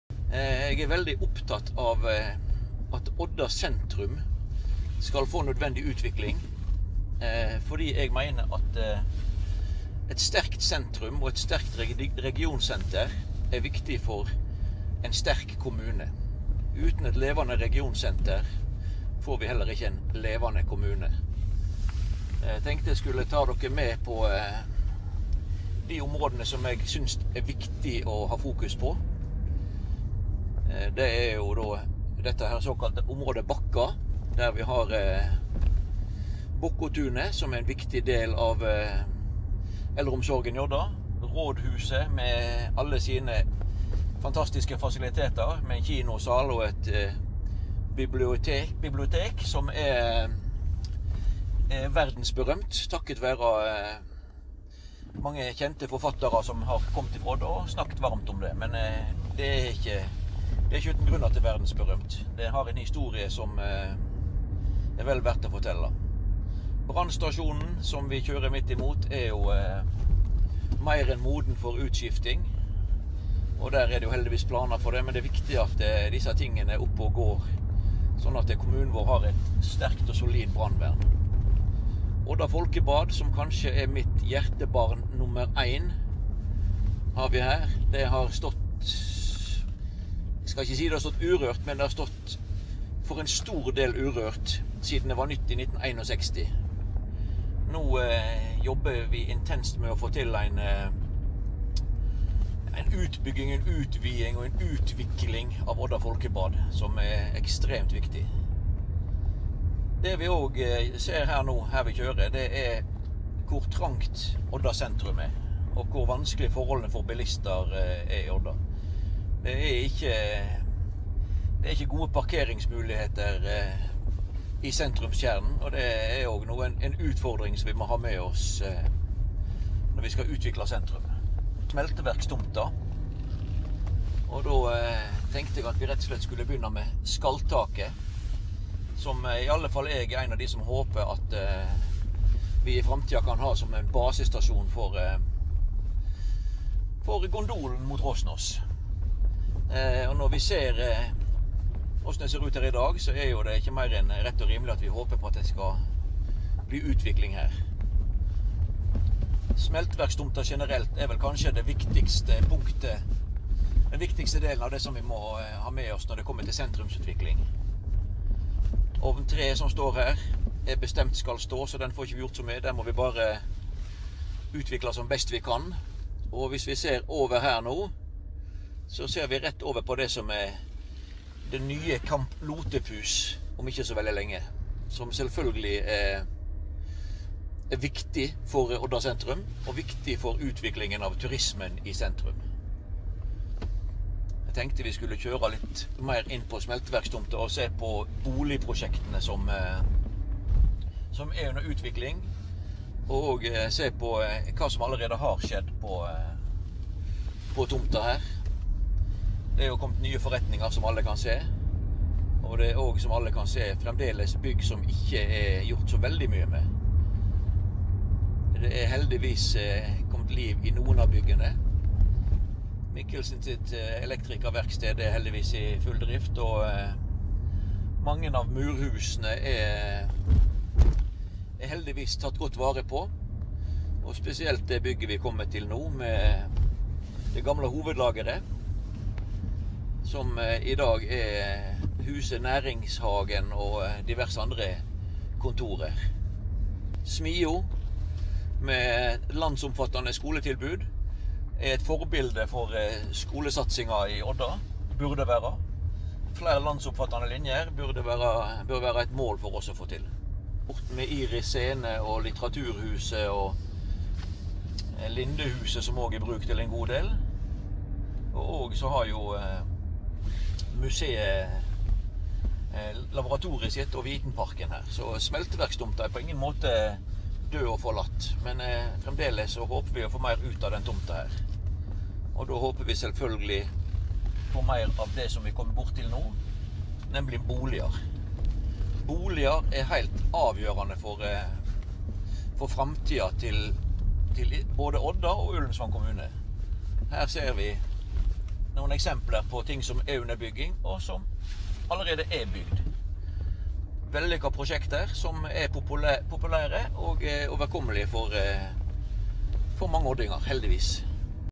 Kommunevalg 2023 - presentasjon vidar Ullensvang AP
Et forsøk på å visualisera noe av det eg e opptatt av som lokalpolitikar i Ullensvang